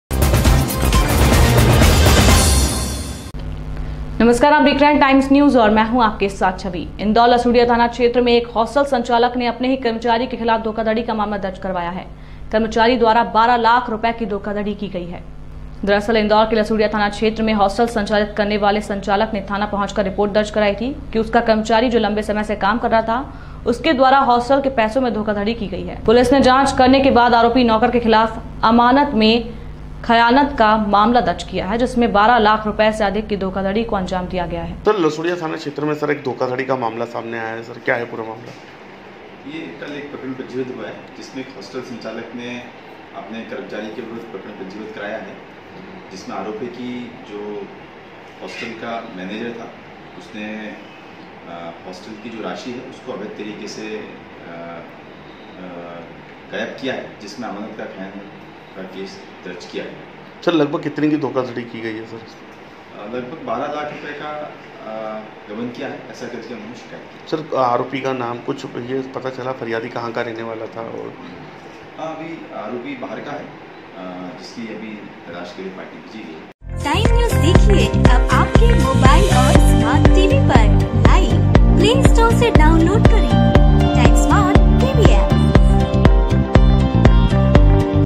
हॉस्टल कर्मचारी ने की हॉस्टल संचालक से लाखो की धोखाधड़ी
इंदौर लसूड़िया थाना क्षेत्र में एक हॉस्टल संचालक ने अपने ही कर्मचारी के खिलाफ धोखाधड़ी का मामला दर्ज करवाया है कर्मचारी द्वारा करीब 12 लाख रुपए की धोखाधड़ी की गई थी।

दरअसल इंदौर के लसूड़िया थाना क्षेत्र में हॉस्टल संचालित करने वाले संचालक ने थाने पहुंचकर रिपोर्ट दर्ज कराई थी कि उनका कर्मचारी जो लंबे समय से काम कर रहा था उसके द्वारा हॉस्टल के पैसों में धोखाधड़ी की गई है पुलिस ने जांच करने के बाद आरोपी नौकर के खिलाफ अमानत में खयानत का मामला दर्ज किया है जिसने 1200000 रुपए से अधिक की धोखाधड़ी को अंजाम दिया है

राजेश रघुवंशी additional SP